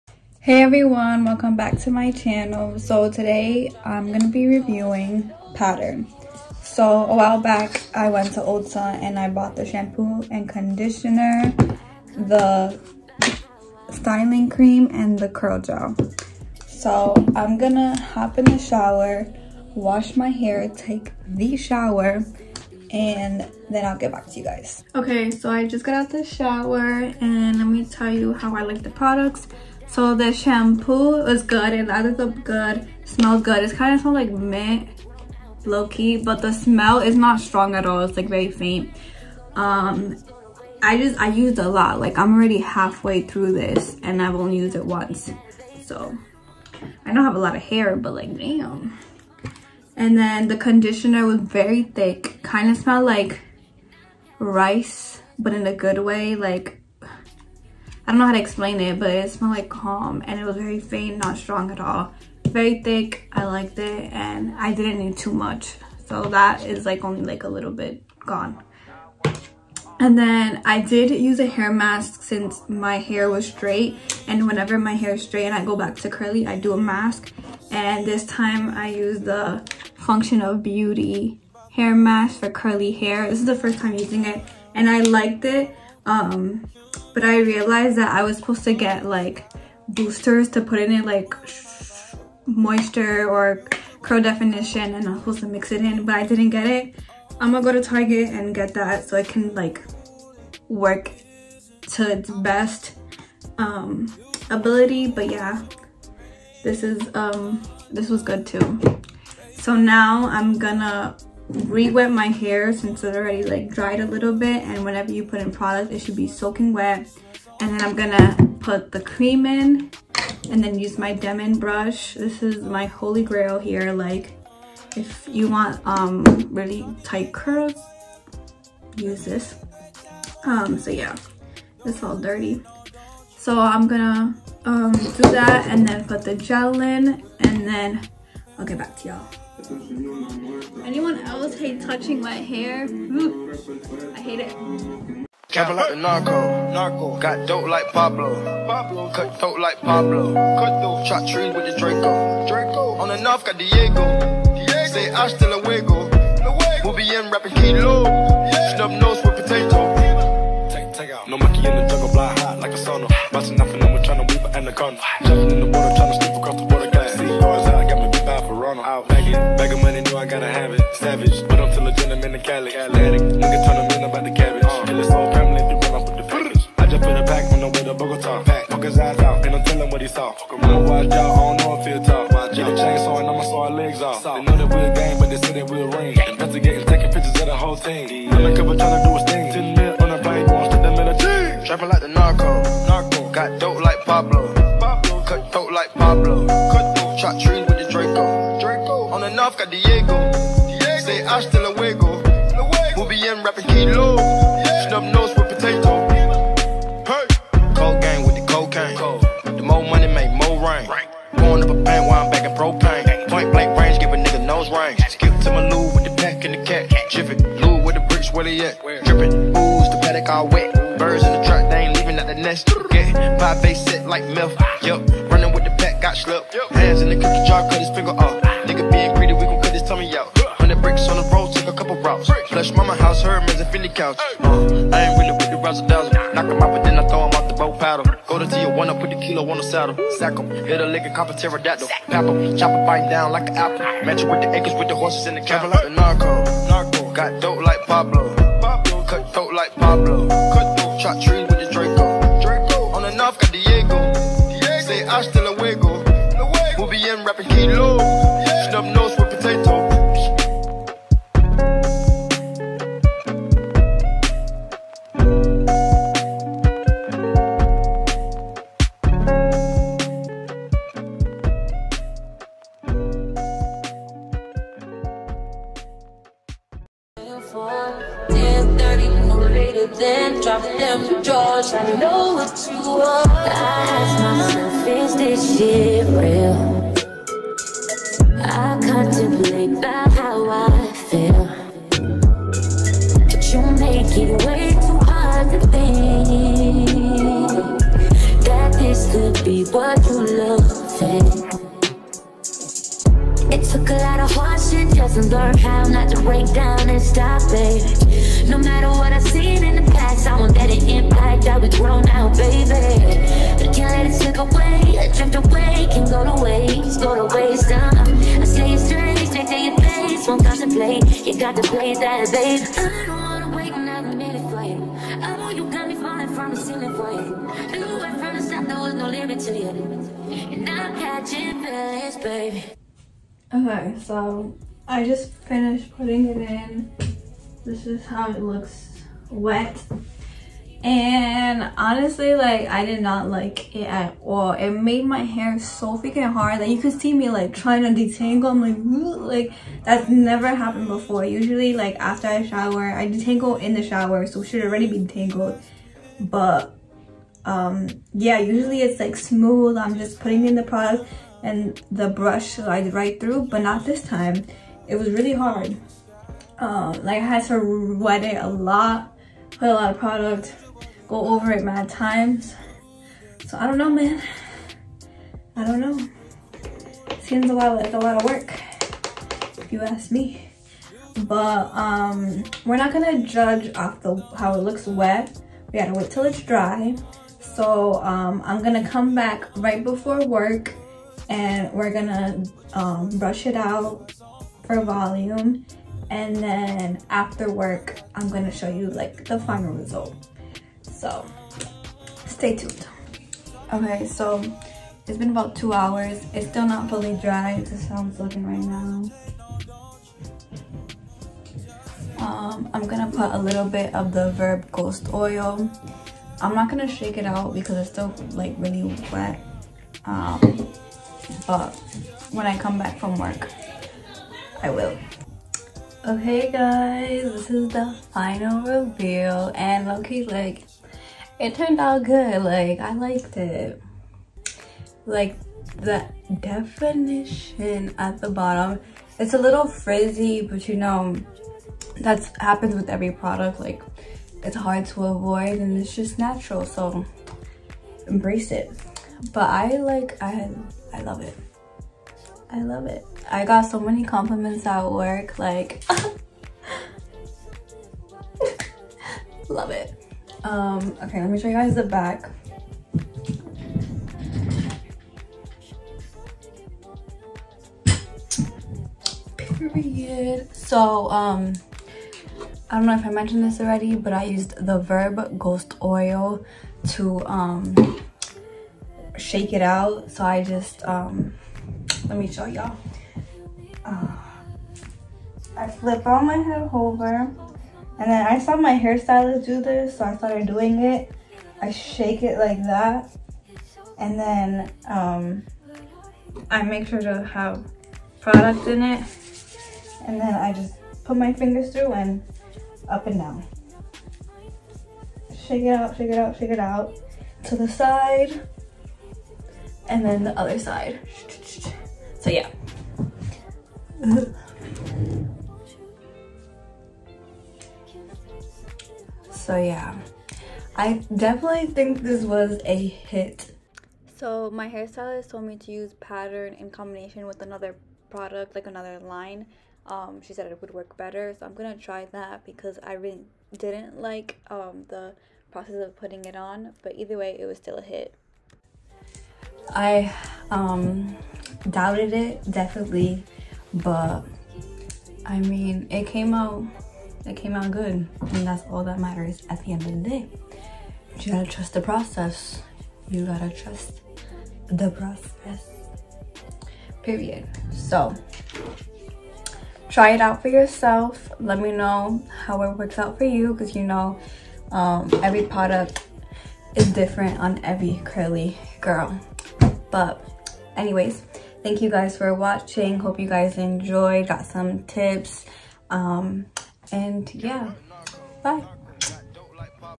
Pattern Product Review | Hit or Miss?
Hi my loves! In today's video I went over my first impressions of Pattern. The good and the bad!